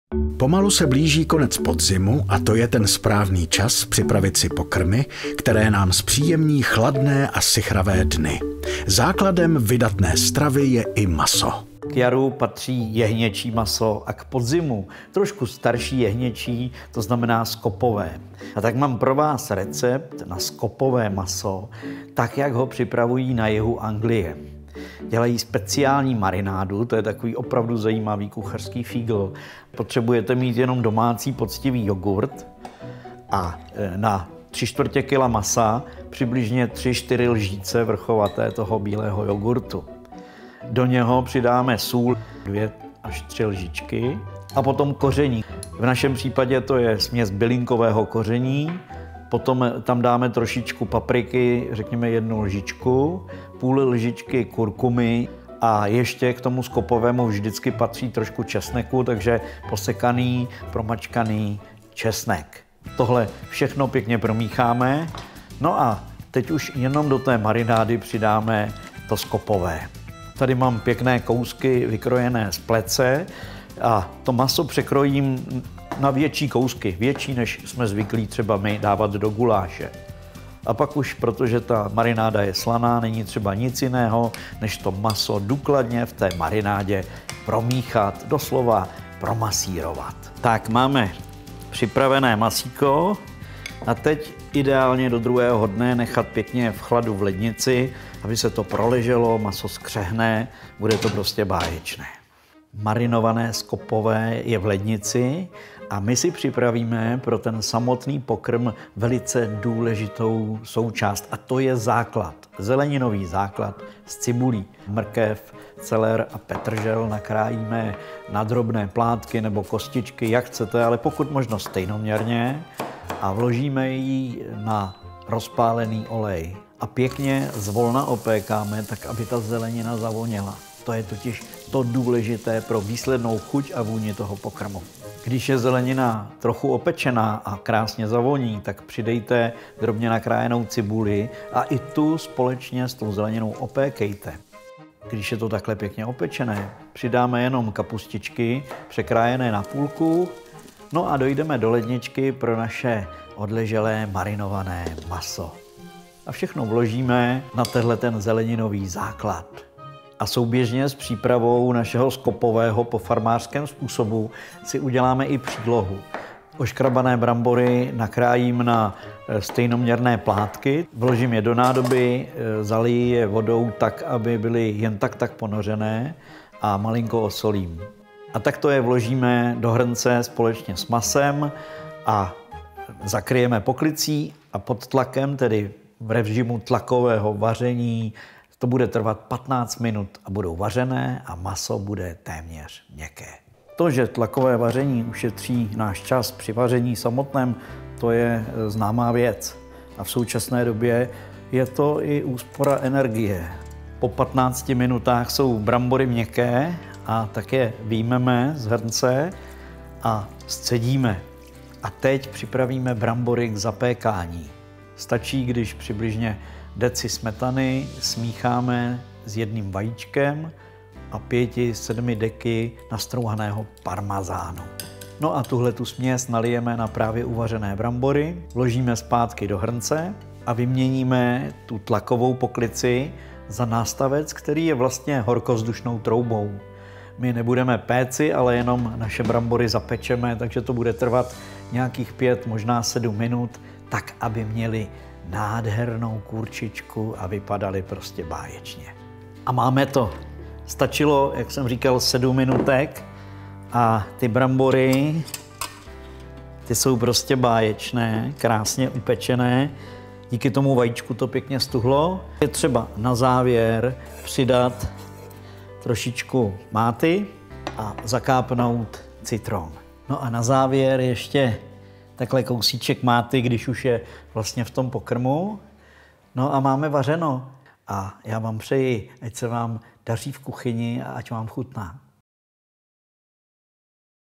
Salente Ario+ - Recept Petra Stupky z jihu Anglie na skopové maso se zapečenými bramborami
Petr má pro vás recept na marinované skopové maso tak, jak ho připravují na jihu Anglie, se speciální marinádou. už se mi sbíhají sliny. Dobrou chuť!

Ingredience:
700g skopového masa
bílý jogurt
kořenová zelenina
brambory
smetanu
vejce
parmazán
směs bylinkového koření
mátu
citron
mletá paprika
kurkumu
česnek
kapustičky
cibuli
olej
sůl